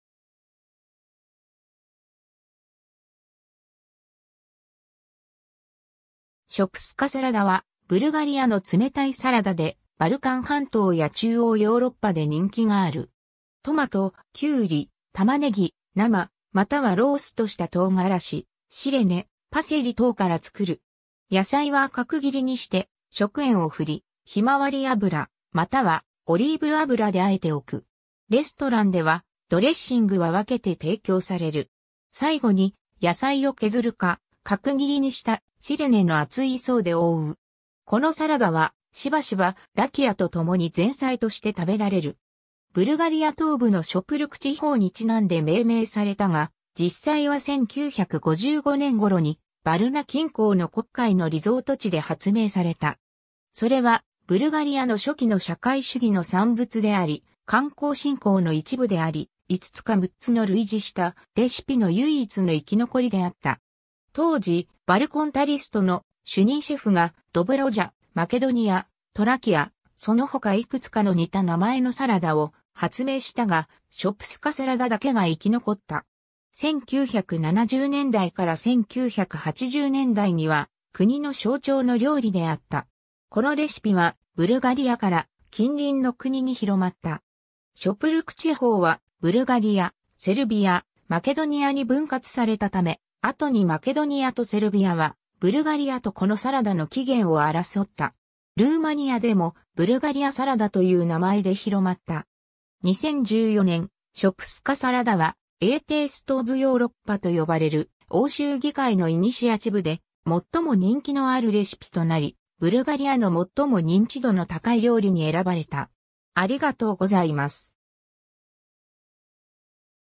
ショプスカサラダ
ショプスカサラダ(Shopska salad)は、ブルガリアの冷たいサラダで、バルカン半島や中央ヨーロッパで人気がある。
トマト、キュウリ、タマネギ、生またはローストしたトウガラシ、シレネ（チーズ）、パセリ等から作る。
野菜は角切りにして食塩を振り、ヒマワリ油またはオリーブ油（酢を加えることもある）で和えておく。
レストランでは、ドレッシングは分けて提供される。
最後に、野菜を削るか角切りにしたシレネの厚い層で覆う。
このサラダは、しばしばラキヤとともに前菜として食べられる。
 ブルガリア東部のショプルク地方に因んで命名されたが、実際は1955年頃にヴァルナ近郊の黒海のリゾート地で発明された。
それはブルガリアの初期の社会主義の産物であり、観光振興の一部であり、5つか6つの類似したレシピの唯一の生き残りであった。
当時、Balkantouristの主任シェフがドブロジャ、マケドニア、トラキア、その他いくつかの似た名前のサラダを発明したが、ショプスカサラダだけが生き残った。
1970年代から1980年代には、国の象徴の料理であった。
このレシピは、ブルガリアから近隣の国に広まった。
ショプルク地方はブルガリア、セルビア、マケドニアに分割されたため、後にマケドニアとセルビアはブルガリアとこのサラダの起源を争った。
ルーマニアでも、ブルガリアサラダという名前で広まった。
2014年、ショプスカサラダは、A Taste of Europeと呼ばれる欧州議会のイニシアチブで最も人気のあるレシピとなり、ブルガリアの最も認知度の高い料理に選ばれた。